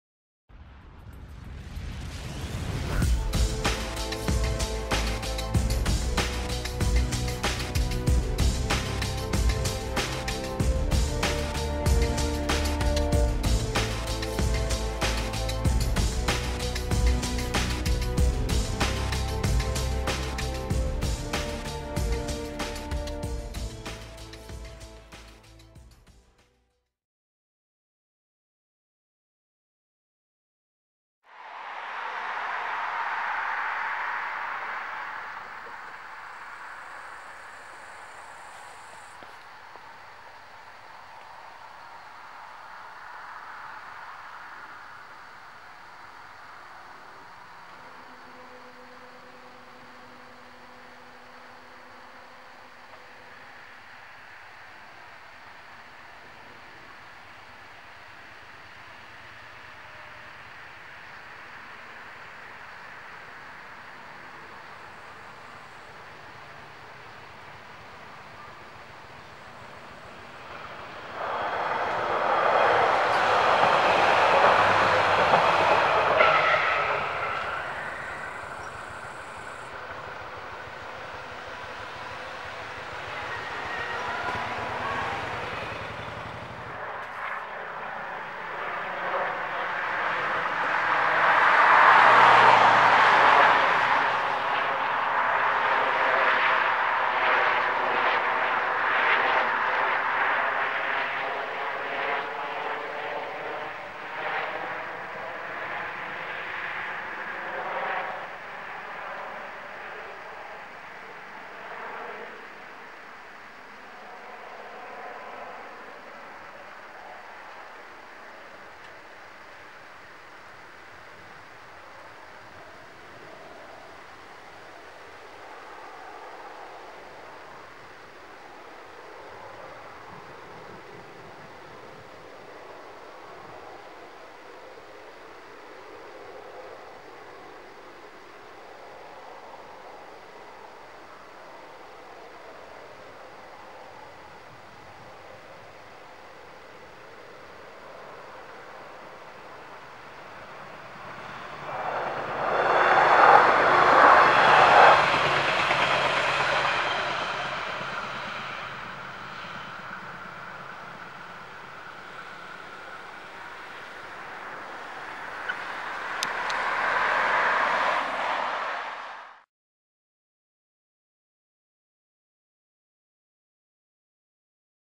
Bahnübergang Krumbach (Schwaben) "Bahnhofstraße"
Krumbach liegt auf halbem Weg zwischen Mindelheim und Günzburg an der Mittelschwabenbahn. Die Bahnstrecke macht im Ortsbereich etliche Bahnübergänge nötig, wobei dieser der erste ist, auf den man trifft, wenn man sich von Norden über die B16 nähert.
Er ist mit Halbschranken und Blinklichtern ausgestattet, von denen 5 von Pintsch Bamag stammen und eines (in Ampeloptik) von Siemens.
Kurioserweise ist das zur Seite gerichtete Licht für einen Feldweg gedacht, der aber nach drei Metern in einer Wiese endet...